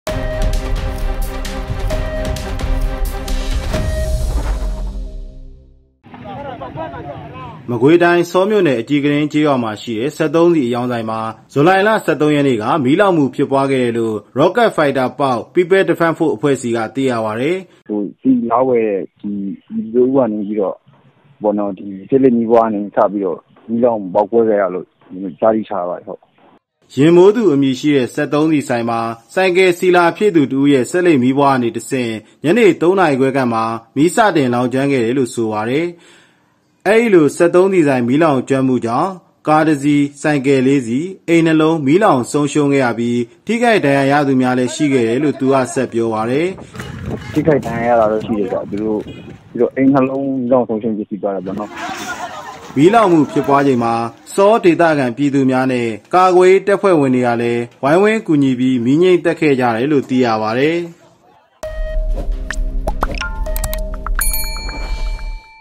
စက်သုံးဆီဆိုင်မှာ ဆီလာဝယ်သူဆေးလိပ်မီးကတဆင့်မီးလောင်မှုဖြစ်ပွားပြီး ကား၊ ဆိုင်ကယ်၊ အိမ် ၂လုံးမီးလောင်
စက်သုံးဆီဆိုင်မှာ ဆီလာဝယ်သူ ဆေးလိပ်မီးကတဆင့် မီးလောင်မှုဖြစ်ပွားပြီး ကား၊ ဆိုင်ကယ်၊ အိမ် ၂ လုံးမီးလောင်

ဆော၊ ဇူလိုင် ၁၄၊၂၀၂၄။

မကွေးတိုင်း၊ ဆောမြို့နယ်၊ အကြည်ကတင်းကျေးရွာမှာရှိတဲ့ စက်သုံးဆီအရောင်းဆိုင်မှာ ဇူလိုင် ၁၃ ရက်က မီးလောင်မှုဖြစ်ပွားခဲ့တယ်လို့ Rocket Fighter Pauk People Defence Force အဖွဲ့ဆီက သိရပါတယ်။

ယဥ်မိုးသူ အမည်ရှိ စက်သုံးဆီဆိုင်မှာ ဆိုင်ကယ်ဆီလာဖြည့်သူတဦးရဲ့ဆေးလိပ် မီးပွားကနေတဆင့် ညနေ ၃ နာရီခွဲခန့်မှာ မီးစတင်လောင်ကျွမ်းခဲ့တယ်လို့ ဆိုပါတယ်။

"ဆေးလိပ်မီးပွားကနေ မီးလောင်ခဲ့တာပါ။ ပေါက်ကွဲအားပြင်းတော့ အနားမကပ်ရဲကြဘူး။ ဆိုင်ကယ်တွေ ကားနဲ့အိမ်တွေလည်း မီးလောင်ဆုံးရှုံးခဲ့တယ်" လို့ RFP အဖွဲ့တာဝန်ခံ ကိုလပြည့် ကပြောပါတယ်။

အဲ့ဒီလို စက်သုံးဆီဆိုင် မီးလောင်ကျွမ်းမှုကြောင့် ကား ၁ စီး၊ ဆိုင်ကယ် ၄ စီး၊ အိမ် ၂ လုံးမီးလောင်ဆုံးရှုံးခဲ့ရပြီး ထိခိုက်ဒဏ်ရာရသူများလည်း ရှိခဲ့တယ်လို့ သူကဆက်ပြောပါတယ်။

မီးလောင်မှုဖြစ်ပွားချိန်မှာ ဆောဒေသခံပြည်သူများနဲ့ ကာကွယ်ရေးတပ်ဖွဲ့ဝင်တွေကလည်း ဝိုင်းဝန်းကူညီပြီး မီးငြှိမ်းသတ်ခဲ့ကြတယ်လို့ သိရပါတယ်။

သတင်း - စောကျော်ငြိမ်း၊ တည်းဖြတ် - ငြိမ်းဇော်